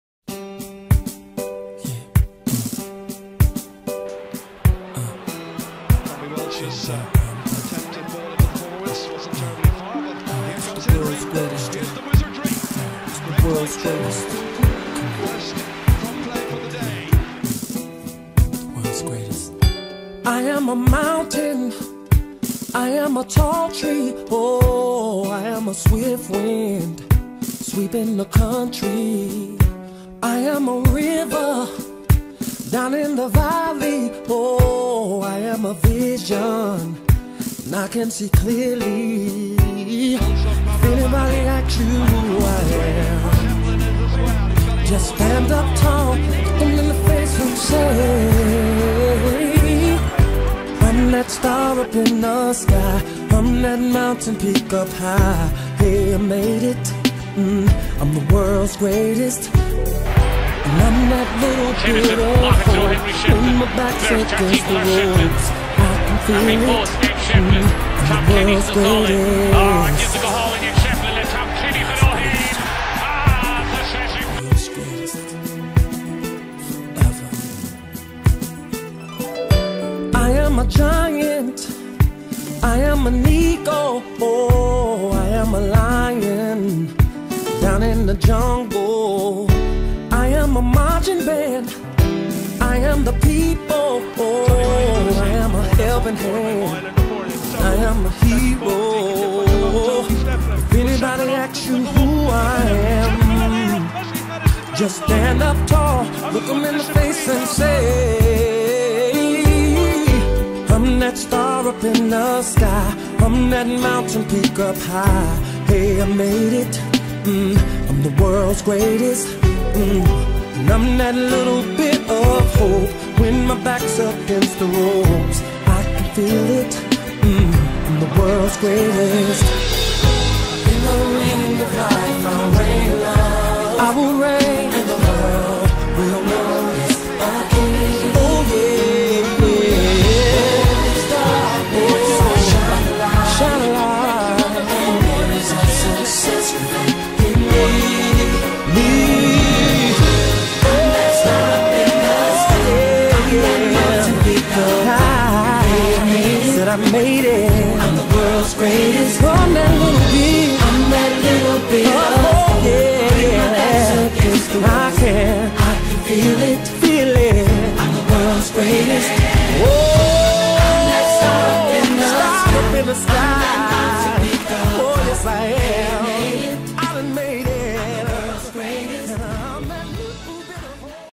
Henry Shefflin Tribute
Ballyhale and Kilkenny hero, Henry Shefflin is arguably the greatest hurler of all time and is the holder of 10 All Ireland Senior Hurling medals. Wow!

Keep up to date with all Hurling news on our facebook